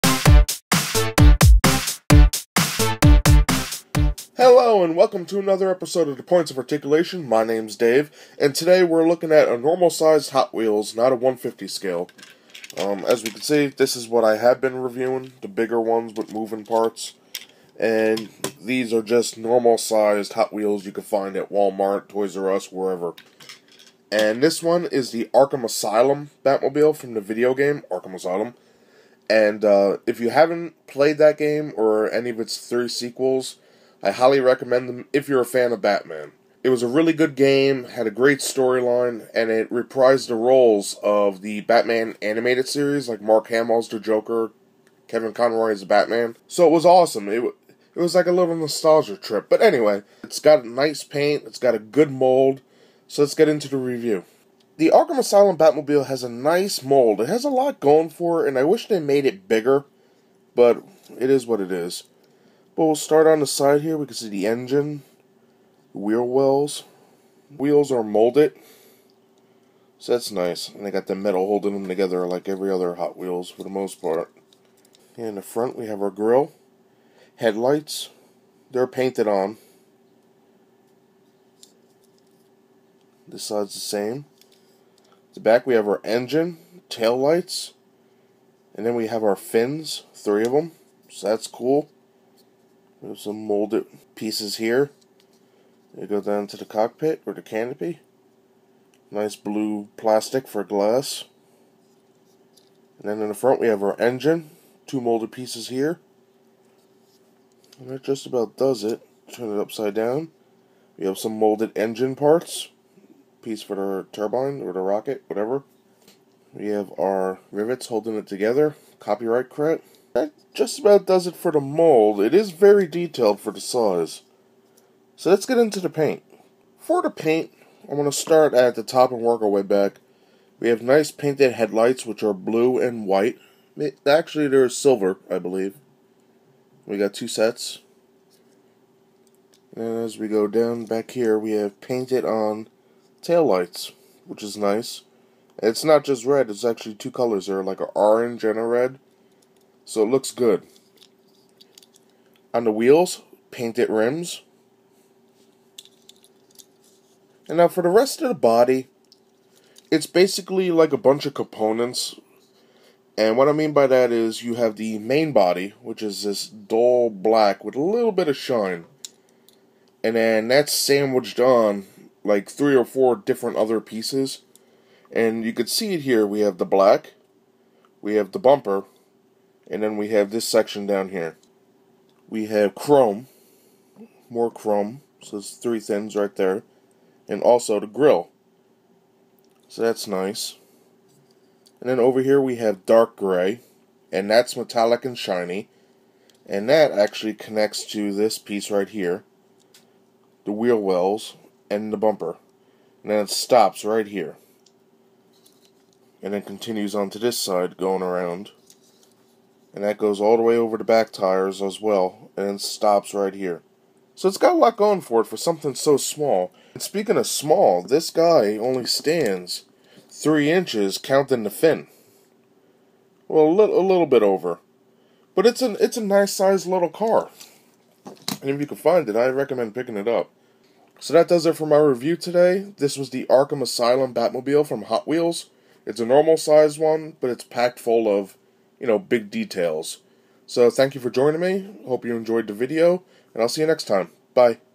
Hot Wheels Arkham Asylum Batmobile
This is not a 1:50 scale car it is just a normal sized toy car. But it is die cast, and I am a huge fan of the Arkham Batman games. When I found out Hot Wheels made one I had to pick it up.